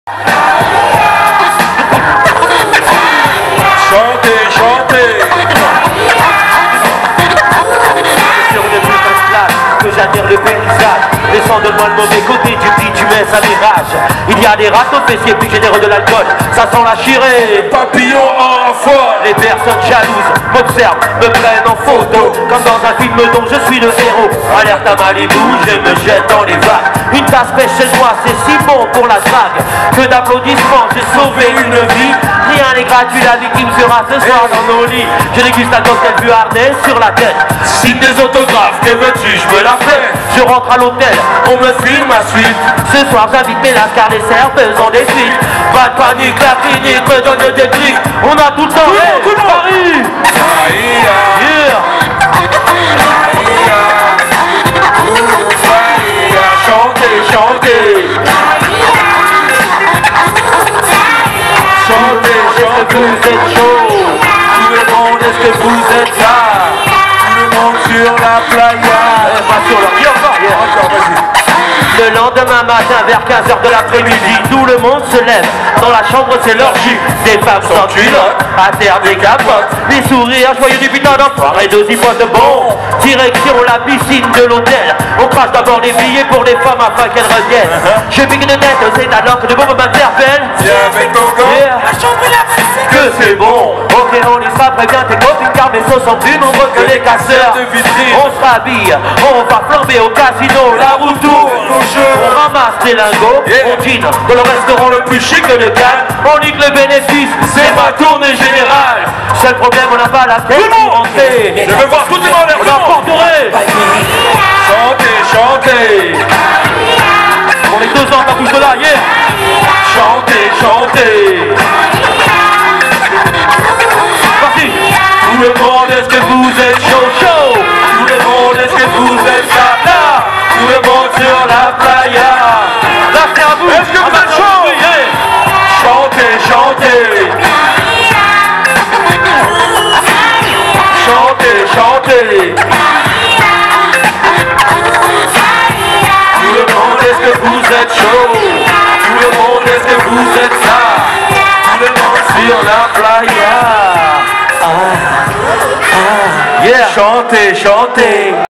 La Clinique Playa Le Grand Festival 2018 au Palais de la Porte Dorée
Grand festival contre le racisme et l'antisémitisme 2018 au Palais de la Porte Dorée Paris

Vendredi 23 mars 2018, Passi, Neg'Marrons, Cut Killer, 2 bal, Driver & DJ Noise débarquent Palais de Porte Dorée pour retracer l'histoire du hip hop français à travers ses plus gros classiques dans un showcase live !